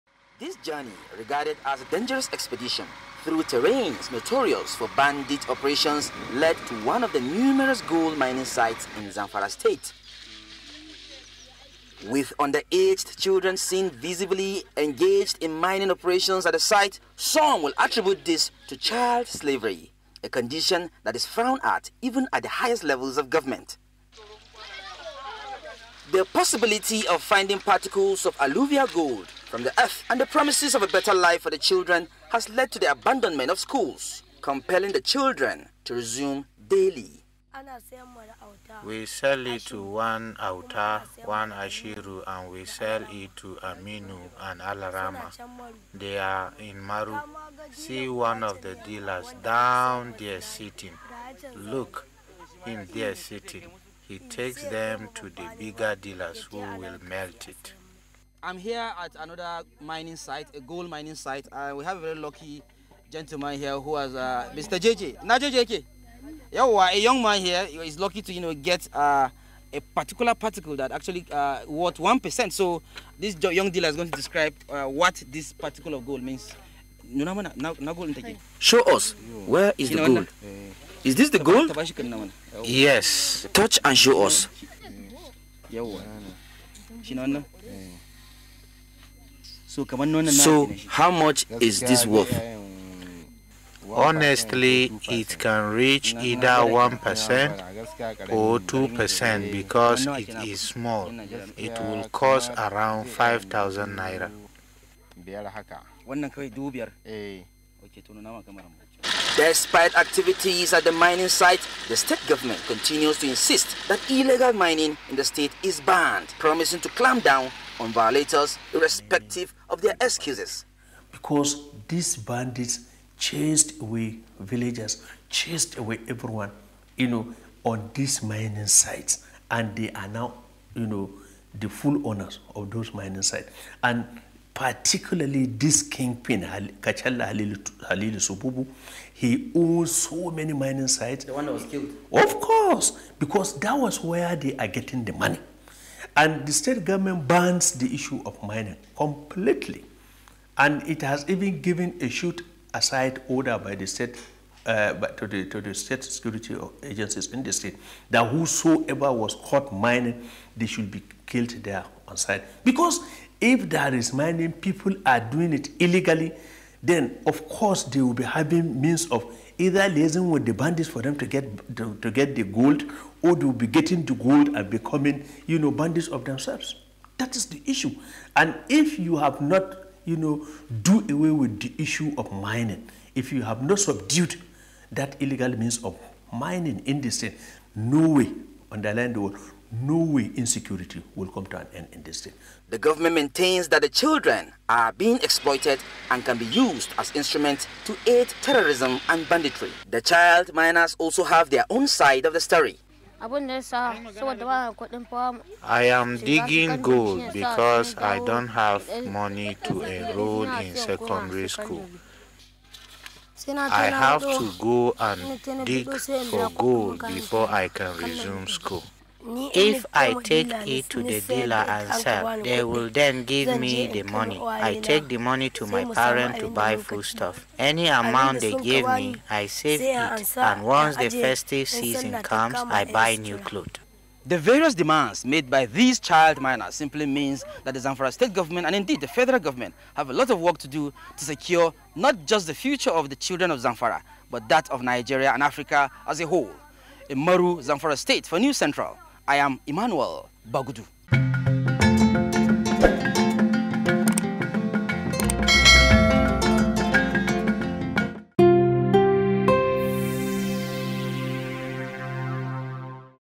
Zamfara State Maintains Mining Ban as Child Labor Exposed
the Zamfara State government has reaffirmed its shoot-at-sight directive on illegal mining activities despite rising concerns over child miners. News Central Television's exclusive report reveals that children and youths are increasingly involved in these dangerous practices, driven by the economic hardship gripping the region. The shocking revelation raises critical questions about the impact of poverty on vulnerable communities and the effectiveness of the state's response to illegal mining.